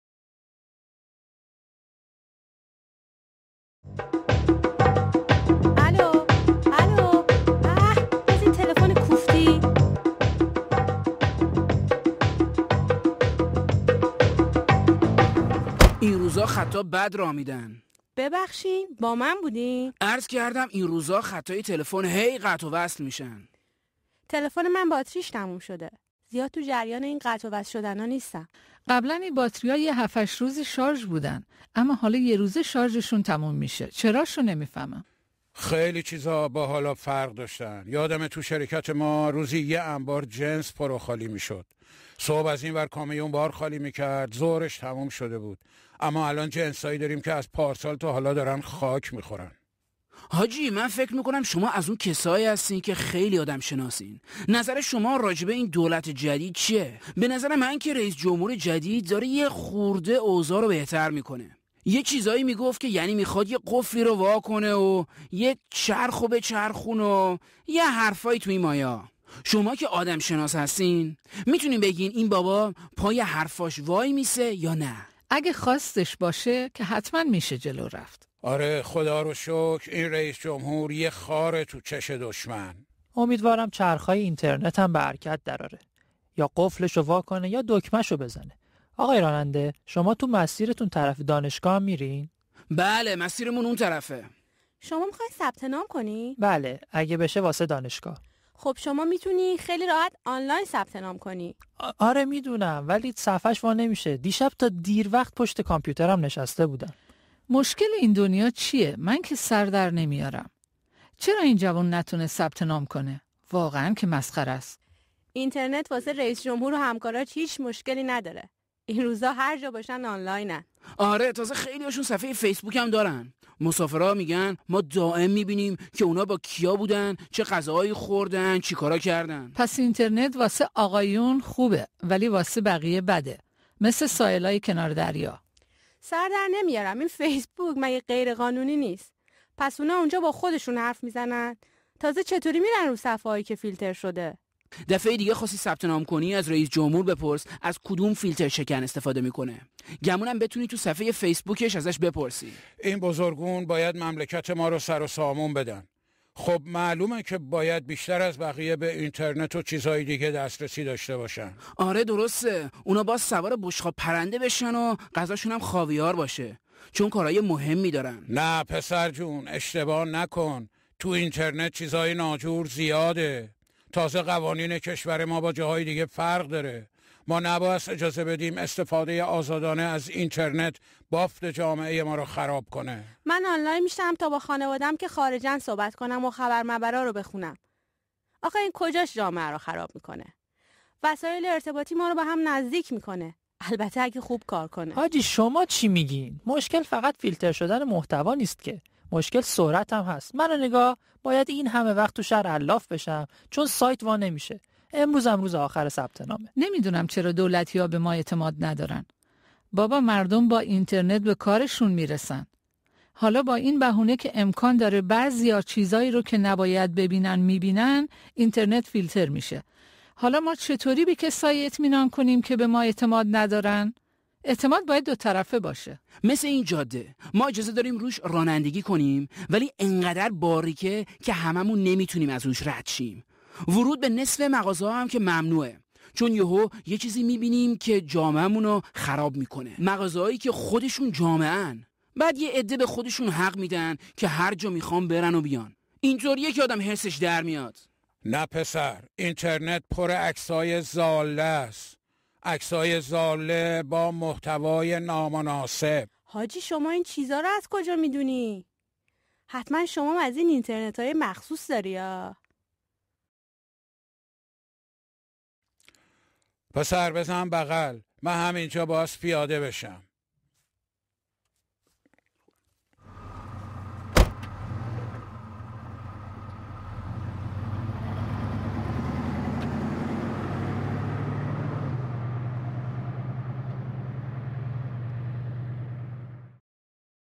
سانسور اینترنت در ایران: تاکسی در بحث
سانسور اینترنت در ایران بر کسی پوشیده نیست. چه سایت‌های مسدود شده باشد، چه کلیدواژه های فیلتر شده یا محدودیت روی پهنای باند، دسترسی به اینترنت معمولا با چالش روبرو است. ضرر ارائه اینترنت بدون محدودیت و سرعت‌بالا به شهروندان ایرانی در چیست؟ امروزه رهبران ایران آزادانه از طریق شبکه‌های اجتماعی با عموم مردم ارتباط برقرار می‌کنند، همان شبکه‌هایی که دسترسی به آنها برای شهروندان عادی مجاز نیست. در این قسمت از مجموعه تاکسی در بحث، سرنشینان تاکسی از نگاه زندگی روزمره‌شان به بحث درباره دسترسی به اینترنت در ایران و یک بام و دو هوائی سانسور اینترنت می‌پردازند.
زبان:‌ فارسی با زیرنویس انگلیسی

Internet censorship in Iran is a well-known issue. Whether blocking websites, filtering keywords or throttling bandwidth, Internet access in Iran can be challenging. What is the harm in allowing Iranian citizens unblocked and high speed Internet access? Iranian leaders now openly connect with the public via social media outlets, even when these same social media platforms are blocked for ordinary citizens. In this taxi talk segment, passengers and the driver in a shared taxi cab discuss Internet access within the context of their daily lives and the bizarre dichotomy of Internet censorship. Language: Farsi with English subtitles.